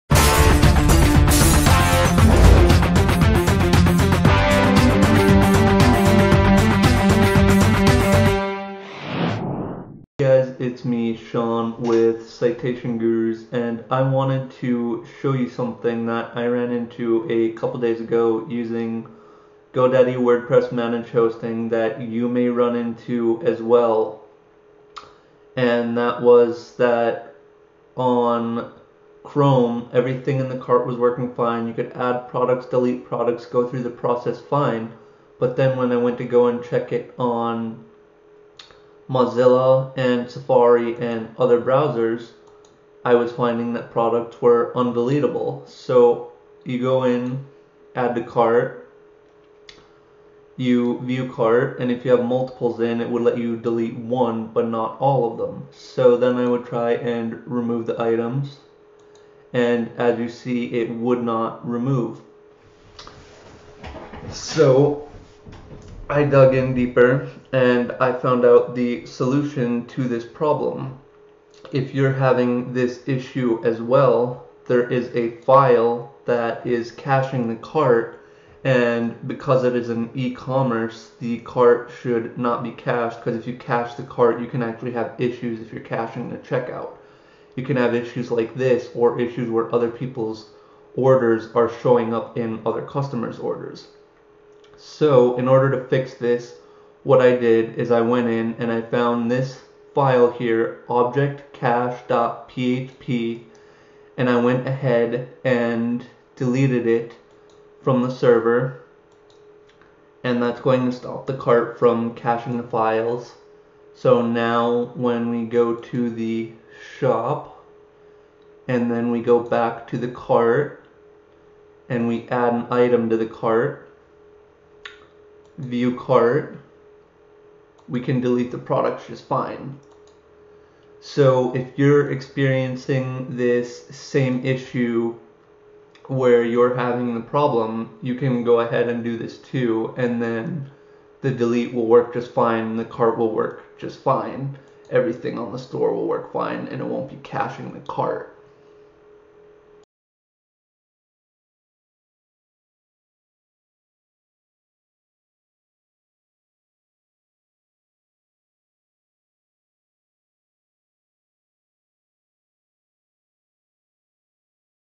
woocommerce undeletable items in cart
In this video I will show you how to deal with Woocommerce items not being deletable in Mozilla, Safari and other browsers. This can happen if you are caching your cart as you are caching the page and the items so when you try to delete you get the cached page again with the items in your cart.

This can also cause issues where other customers can end up with the wrong orders in their cart at checkout. To fix this there is a simple file you need to delete on wordpress managed hosting which will stop the cart from caching and allow your cart to work on all browsers.

This video will show you that file and how to get rid of it so your store will work like it should once again.